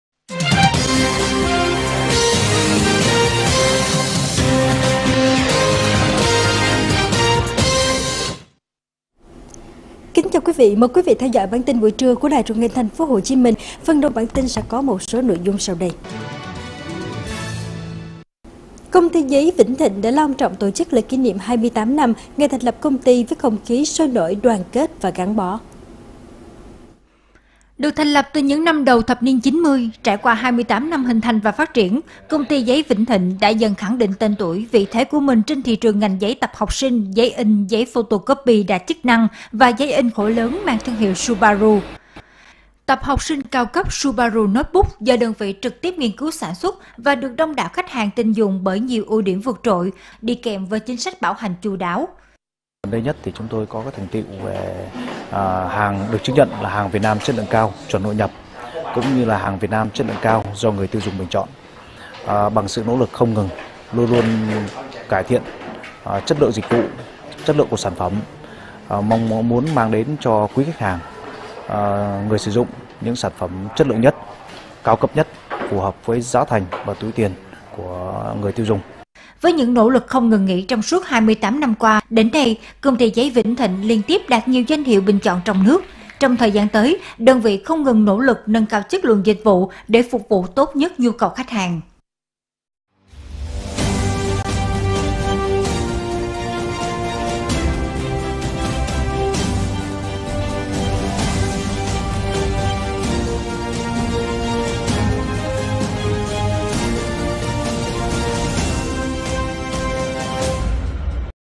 HTV9 ĐƯA TIN CTY GIẤY VĨNH THỊNH LONG TRỌNG TỔ CHỨC 28 NĂM THÀNH LẬP DOANH NGHIỆP.
Được thành lập từ những năm đầu thập niên 90, trải qua 28 năm hình thành và phát triển, Công ty Giấy Vĩnh Thịnh đã dần khẳng định tên tuổi, vị thế của mình trên thị trường ngành giấy tập học sinh, giấy in, giấy photocopy đa chức năng và giấy in khổ lớn do Công ty Giấy Vĩnh Thịnh trực tiếp nghiên cứu sản xuất và được đông đảo khách hàng tin dùng như: Giấy thương hiệu Subaru, Tập học sinh cao cấp Subaru Notebook,… Điểm nổi trội ở các sản phẩm Giấy của Công ty Giấy Vĩnh Thịnh sản xuất chính là không bụi, không bị cuốn hay cong vênh, không bị kẹt vào máy khi in, khi photocopy ở nhiệt độ cao và tốc độ nhanh, giúp Khách hàng tiết kiệm mực từ 15% đến 20% so với các chủng loại giấy khác trên thị trường. Đặc biệt Công ty Giấy Vĩnh Thịnh có chính sách bảo hành từng tờ giấy khi tới tay người tiêu dùng !

Với những nỗ lực không ngừng nghỉ của tất cả công nhân viên trong suốt 28 năm qua, đến nay, Công ty Giấy Vĩnh Thịnh là một trong những nhà cung cấp tiên phong trong ngành Giấy photocopy, Giấy in, Giấy tập học sinh và liên tiếp đạt danh hiệu Top 10 Thương hiệu – Nhãn hiệu tin dùng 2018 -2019”, “Sao Vàng Doanh nhân Đất Việt, Thương hiệu, Nhãn hiệu tin dùng”, đạt chứng nhận “Hàng Việt Nam Chất Lượng cao do người tiêu dùng bình chọn”, “Hàng Việt Nam Chất lượng Cao Chuẩn hội nhập”…

Được sự tín nhiệm của Quý Khách hàng, tiếp nối thành tựu của 28 năm, Công ty Giấy Vĩnh Thịnh luôn liên tục nỗ lực tìm kiếm các nguồn hàng chất lượng ổn định để cung cấp cho Quý Khách các sản phẩm có chất lượng tuyệt hảo nhất và không ngừng nâng cao chất lượng dịch vụ tốt nhất.